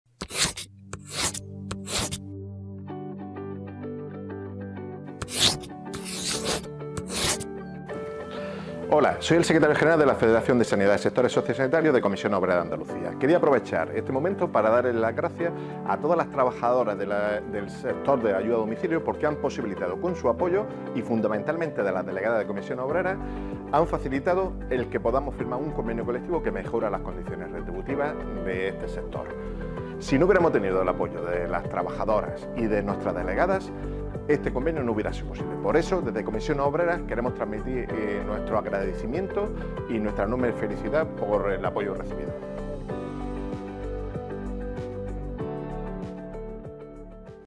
Publicado el I Convenio Colectivo Andaluz de Ayuda a Domicilio
En BOJA nº 181, de 20 de septiembre de 2023, se ha publicado el I convenio colectivo autonómico del sector de ayuda a domicilio, cuya vigencia se extiende hasta el 31 de diciembre de 2027. La firma de este convenio, que beneficia a más de 40.000 personas trabajadoras, aporta estabilidad y mejora la situación laboral de este importante sector.